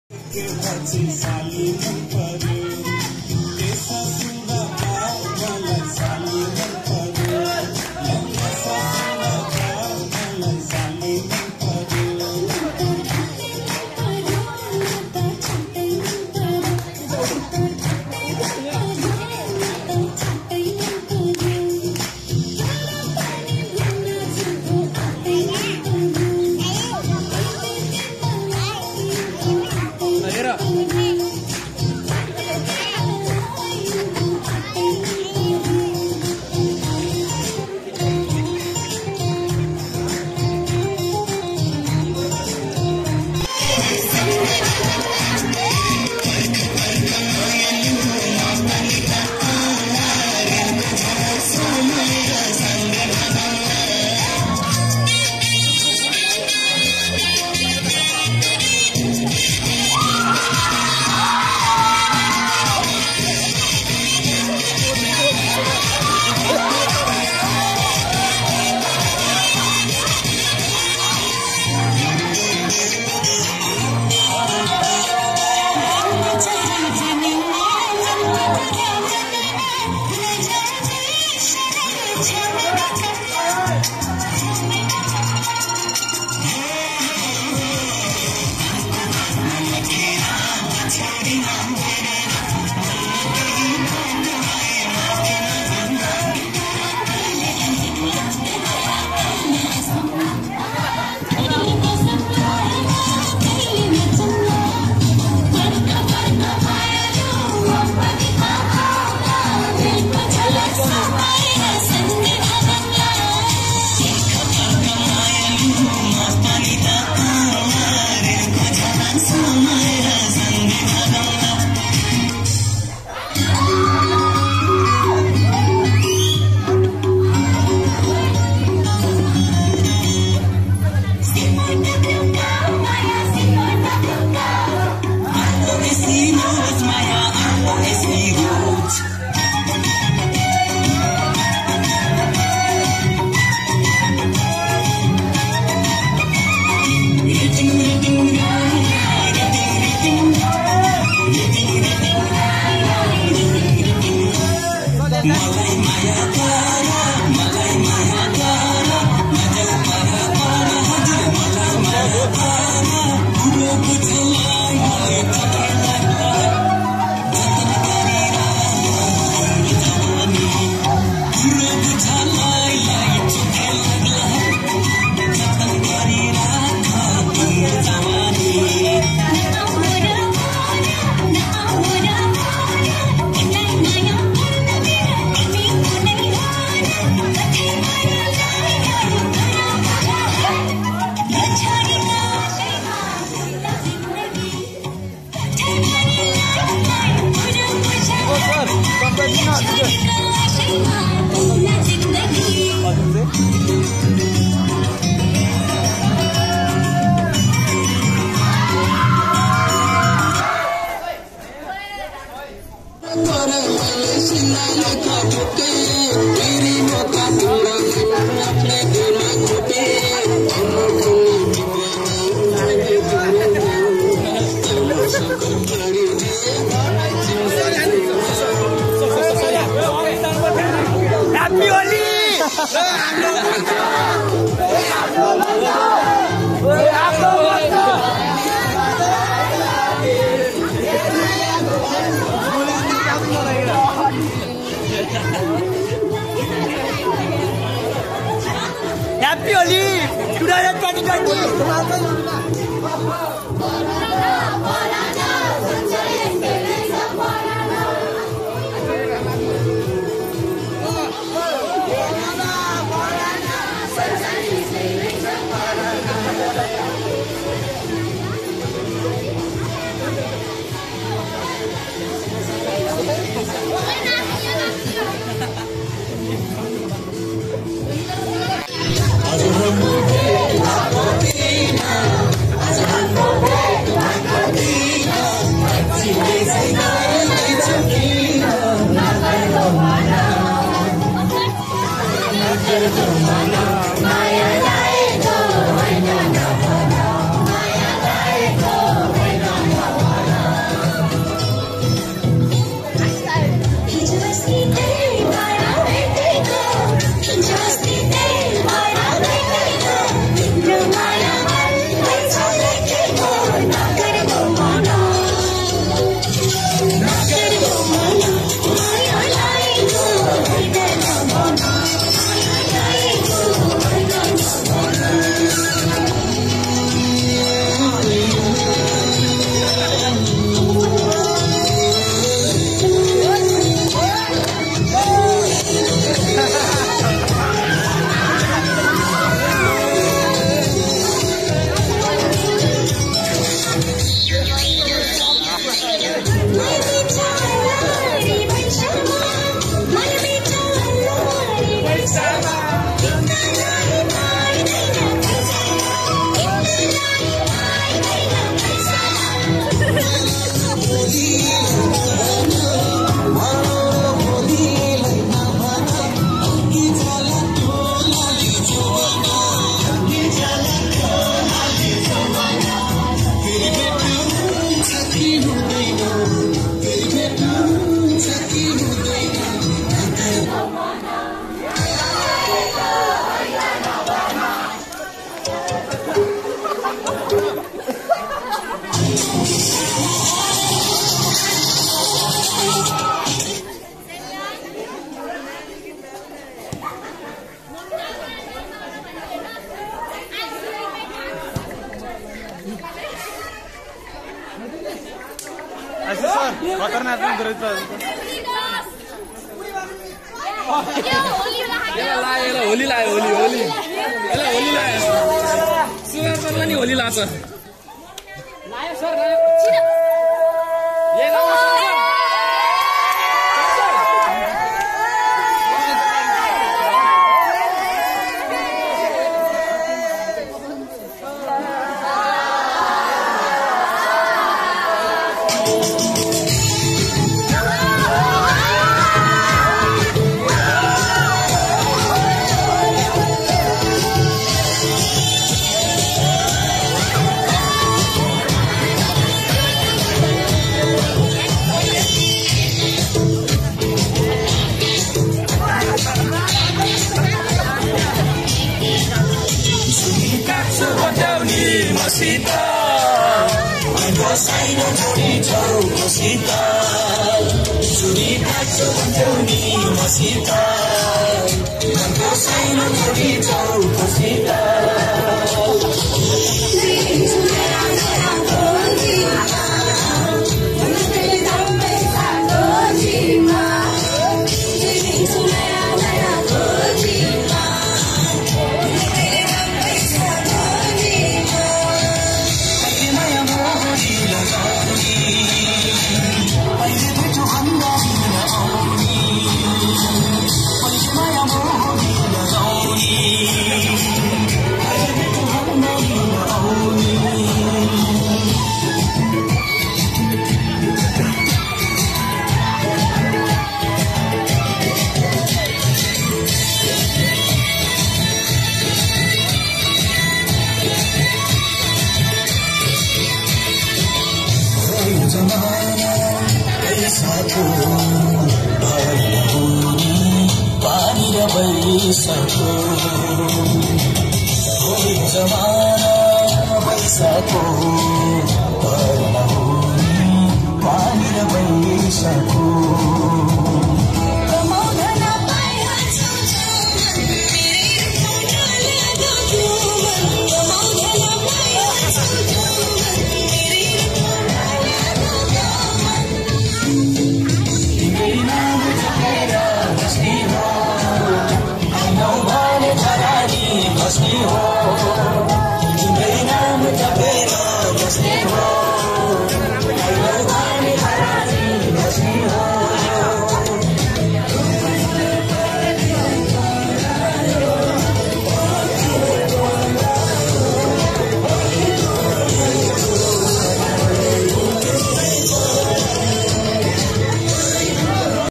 Dance in farewell program
Class 10 students getting farewell program in Marigold school 29 feb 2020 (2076)